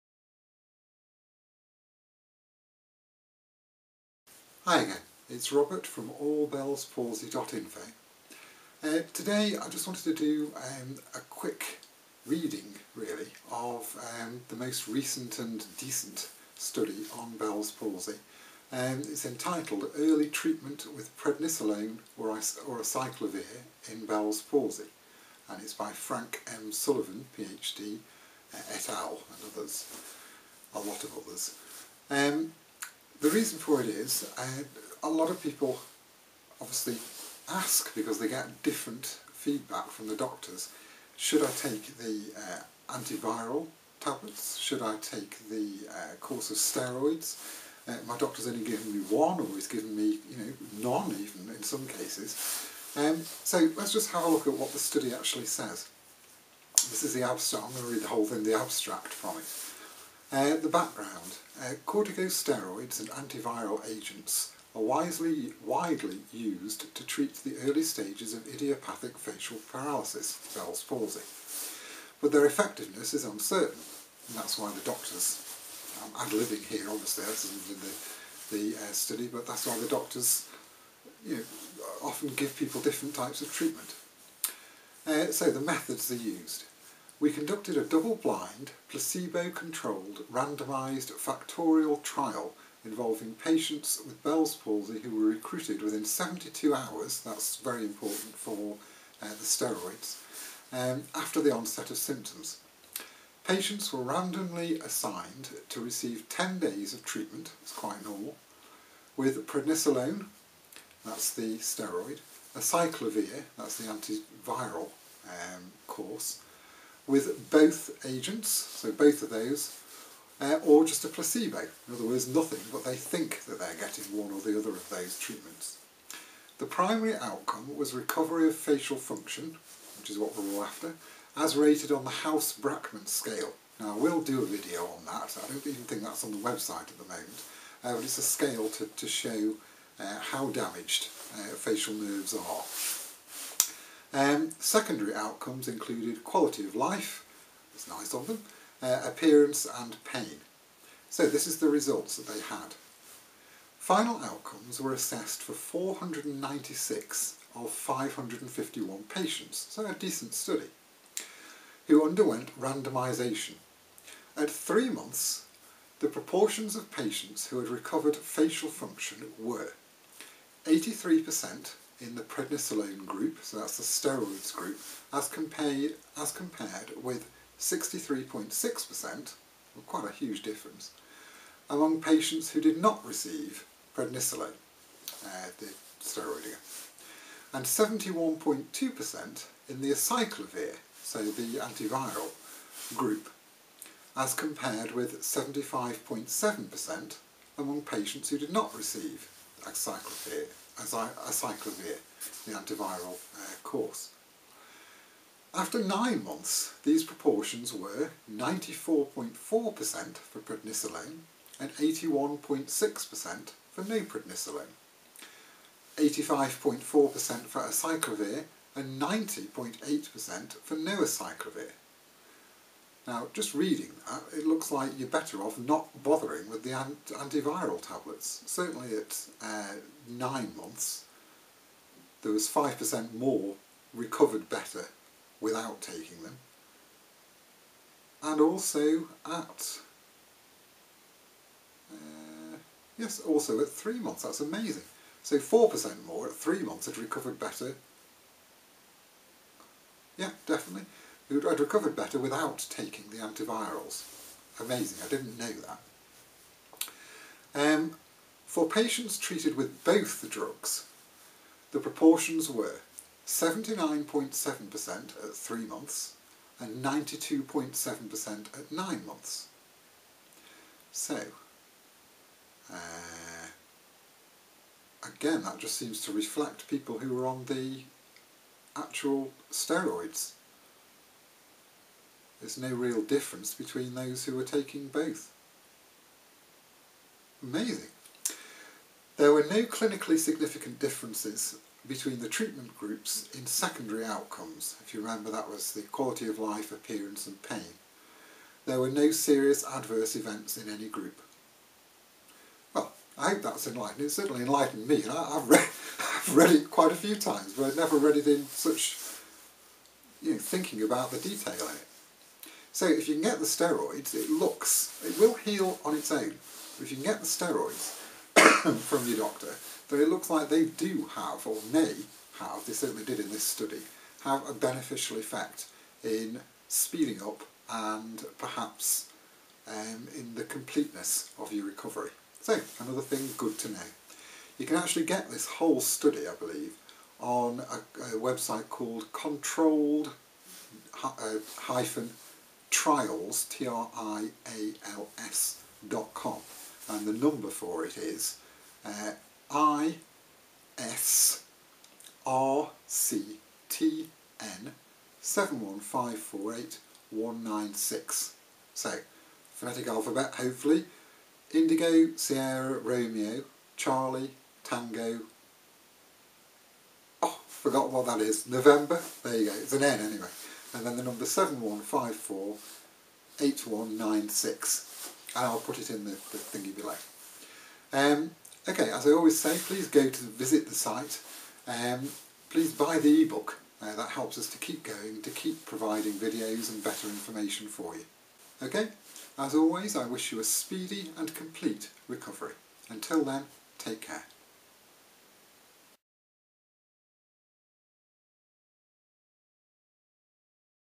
Bells Palsy Treatment - Steroids or Antiviral Study
for the main Bell's Palsy Information Site. To purchase the Bell's Palsy Guide Ebook please go

Early Treatment with Prednisolone or Acyclovir in Bell's Palsy
ABSTRACT
Background Corticosteroids and antiviral agents are widely used to treat the early stages of idiopathic facial paralysis (i.e., Bell's palsy), but their effectiveness is uncertain.
Methods We conducted a double-blind, placebo-controlled, randomized, factorial trial involving patients with Bell's palsy who were recruited within 72 hours after the onset of symptoms. Patients were randomly assigned to receive 10 days of treatment with prednisolone, acyclovir, both agents, or placebo. The primary outcome was recovery of facial function, as rated on the House--Brackmann scale. Secondary outcomes included quality of life, appearance, and pain.

Results Final outcomes were assessed for 496 of 551 patients who underwent randomization. At 3 months, the proportions of patients who had recovered facial function were 83.0% in the prednisolone group as compared with 63.6% among patients who did not receive prednisolone and 71.2% in the acyclovir group as compared with 75.7% among patients who did not receive acyclovir (adjusted P=0.50). After 9 months, these proportions were 94.4% for prednisolone and 81.6% for no prednisolone  and 85.4% for acyclovir and 90.8% for no acyclovir (adjusted P=0.10). For patients treated with both drugs, the proportions were 79.7% at 3 months  and 92.7% at 9 months. There were no clinically significant differences between the treatment groups in secondary outcomes. There were no serious adverse events in any group.

Conclusions In patients with Bell's palsy, early treatment with prednisolone significantly improves the chances of complete recovery at 3 and 9 months. There is no evidence of a benefit of acyclovir given alone or an additional benefit of acyclovir in combination with prednisolone.
(Current Controlled Trials number, ISRCTN71548196 controlled-trials.com )

I wish you a speedy and complete recovery

Robert.